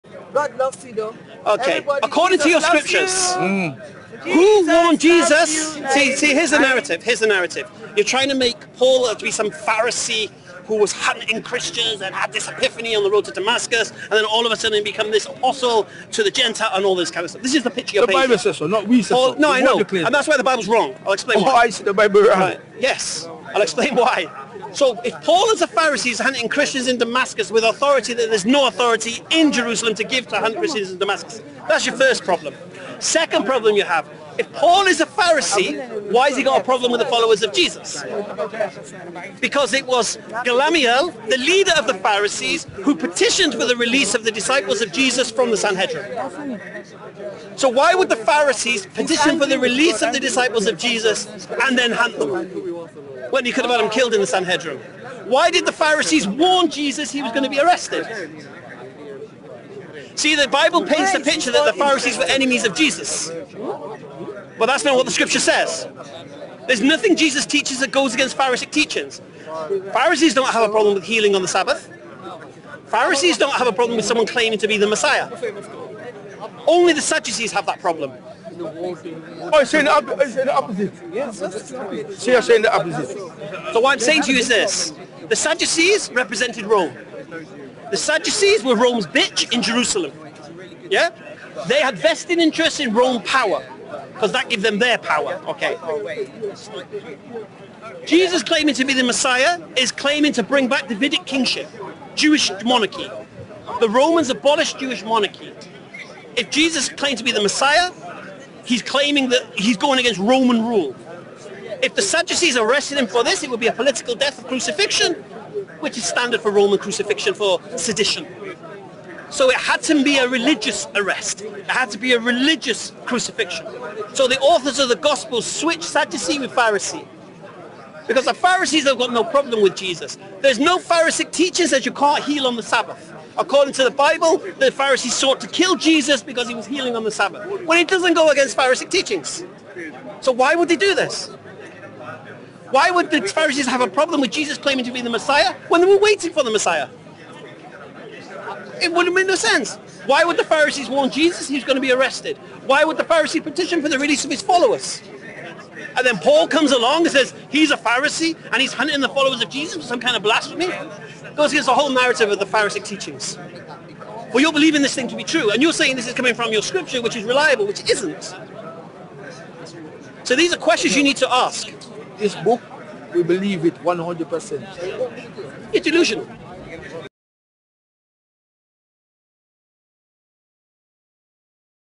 Did authors of the Gospels switch Sadducees with Pharisees ??? Hamza's Den
Why BIBLE is wrong ? explained by Hamza Daren- Paul and Christianity a unique prespective